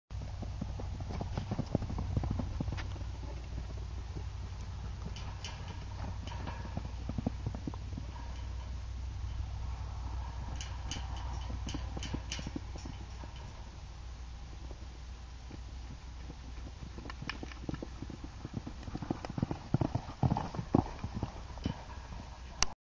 Mary pratice barrels
Mary running barrels at home